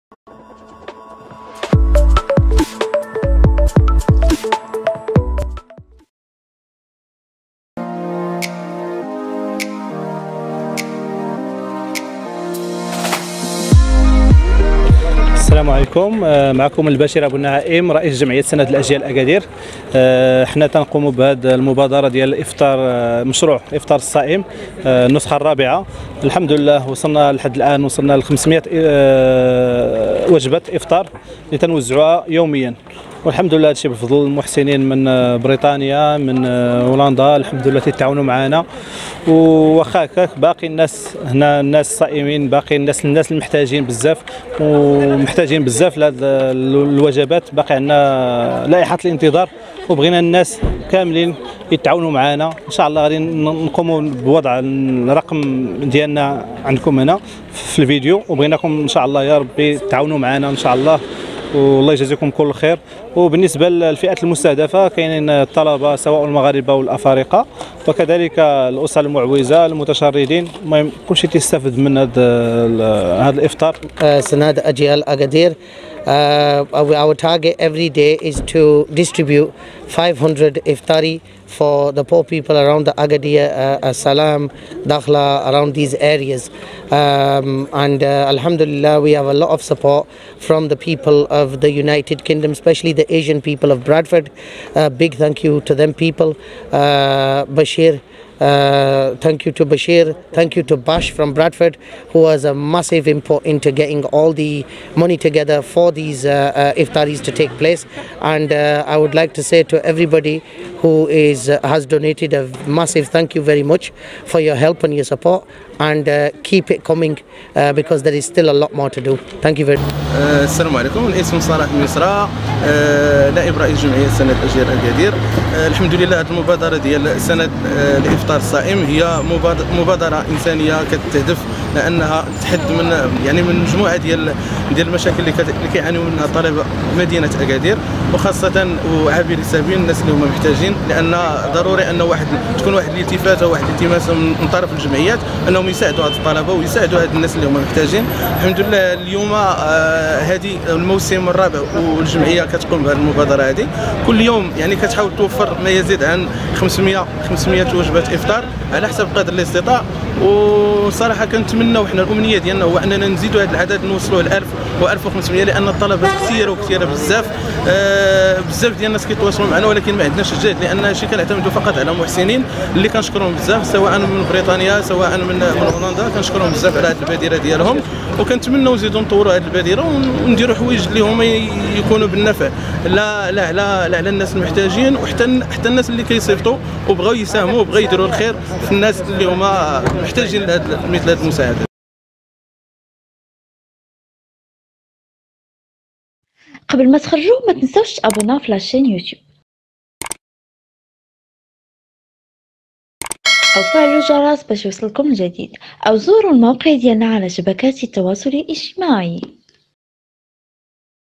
أکادیر : جمعیۃ سند تقوم بتوزیع وجبۃ الإفطار علی الطلبۃ والأسر المعوزۃ
مراسلة : خالد بادو
عدسة : سفيان اهبري

الفايسبوك :

النسخة الفرنسية

 النسخة العربية

عشاق الرياضة

 التويتر

الأنستغرام

البريد الإلكتروني